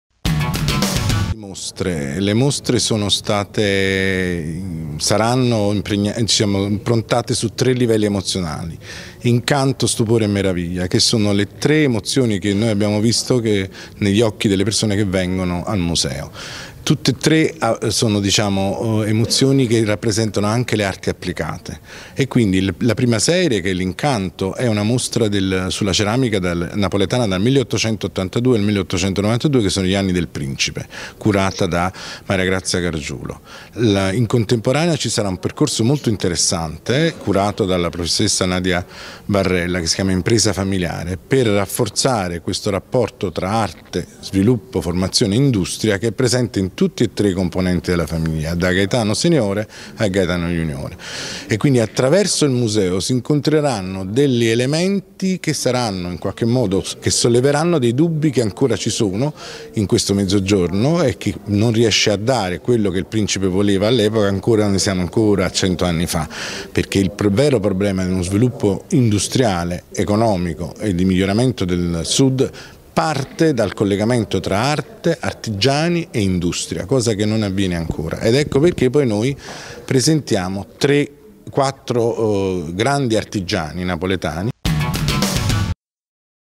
TG 11/04/2017 Guido Cabib
TG 11/04/2017 Guido Cabib - Direttore artistico della mostra
Presentazione mostra “La Primavera del Principe”
Museo civico Filangieri – Via Duomo – Napoli